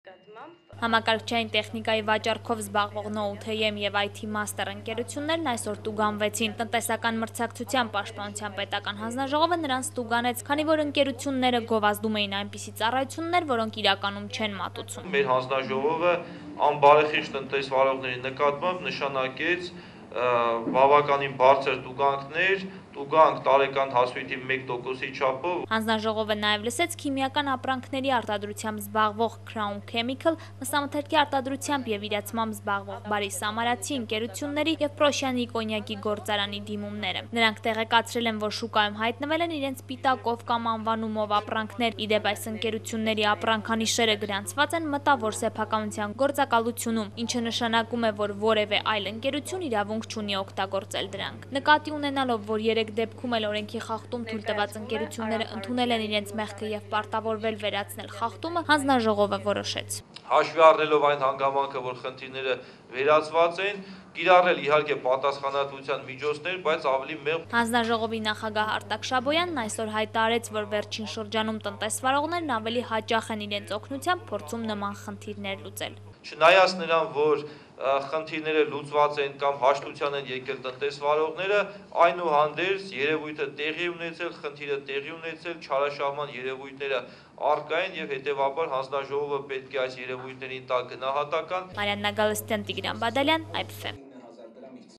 «Nout.am» և «Այ թի մաստեր» ընկերությունները տուգանվեցին
ՀՀ տնտեսական մրցակցության պաշտպանության պետական հանձնաժողովի այսօրվա նիստում հրապարակվեցին համակարգչային տեխնիկայի վաճառքով զբաղվող «Nout.am» և «Այ թի մաստեր» ընկերությունների հանդեպ հարուցված վարչական վարույթների արդյունքները: